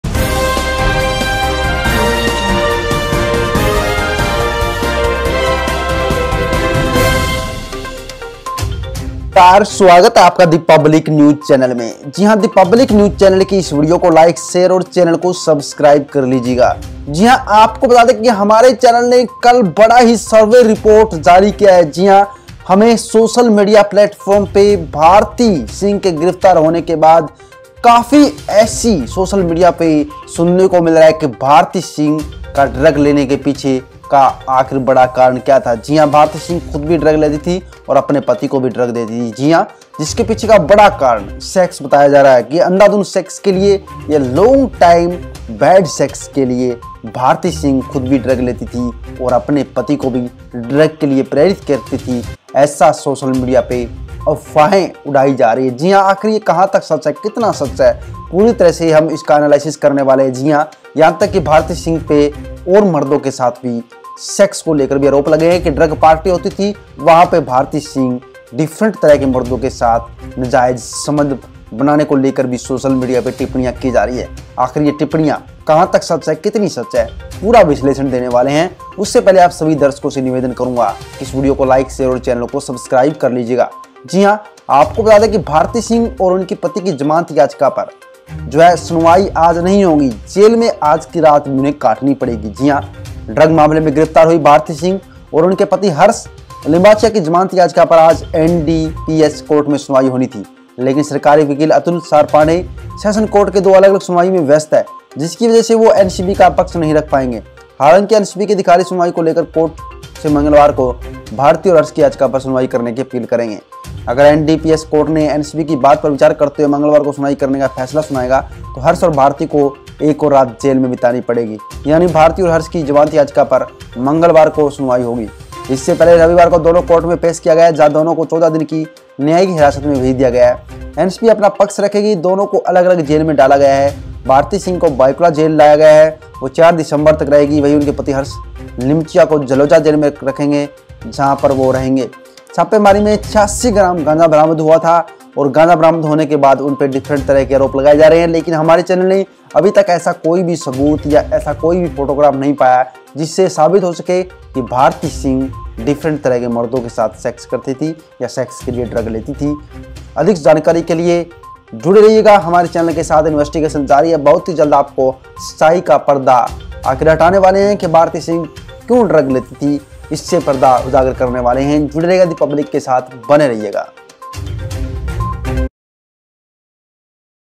भारती सिंह और उनके पति की जमानत याचिका पर नहीं होगी सुनवाई, जेल में कटेगी आज की रात ,The Public
sarkari yojana
sukanya yojana
awass yojana
penshan yojana
kendra sarkar yojana
pm yojana